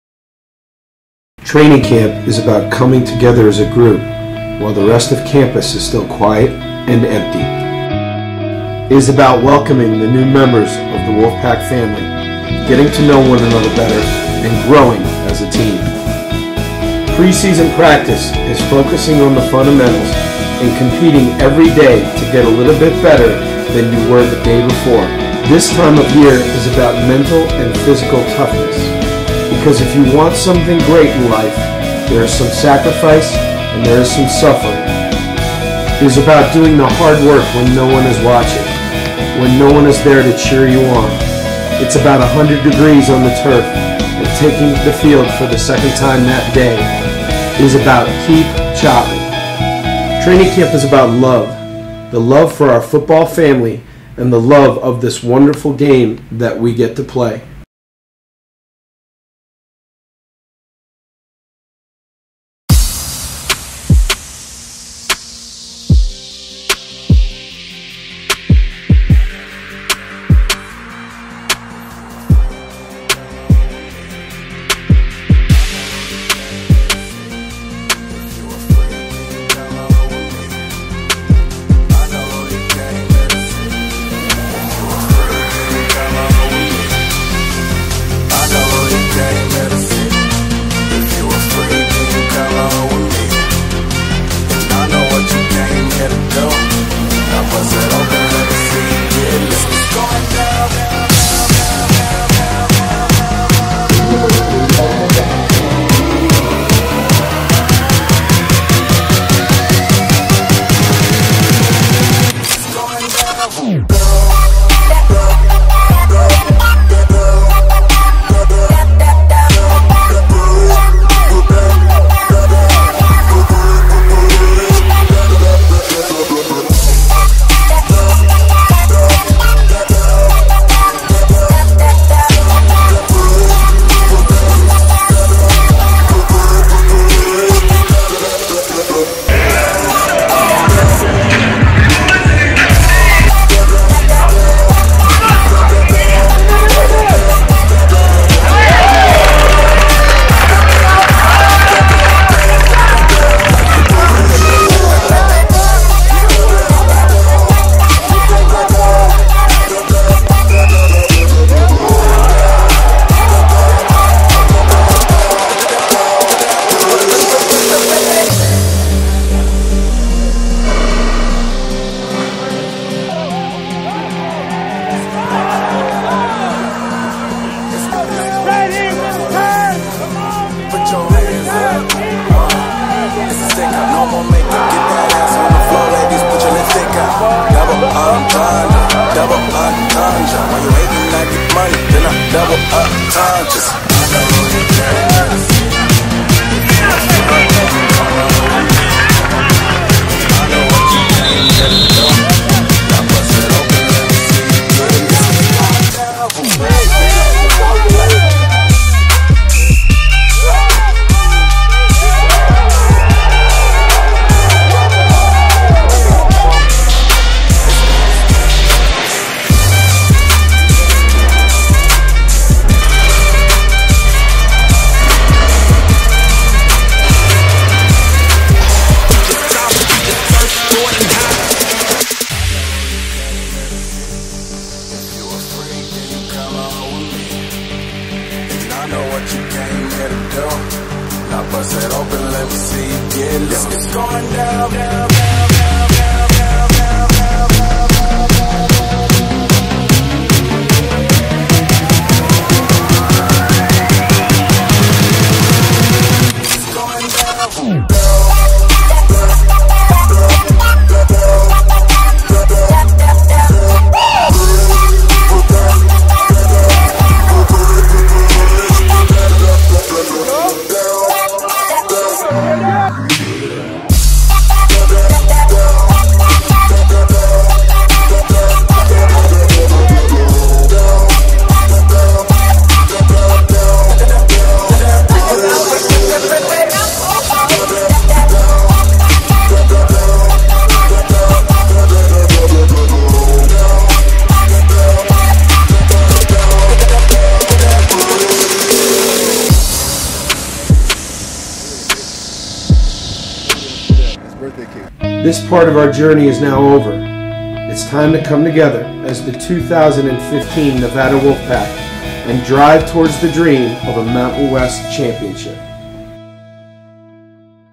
2015 Nevada Football Training Camp
An inside look at the University of Nevada's Football Team Training Camp.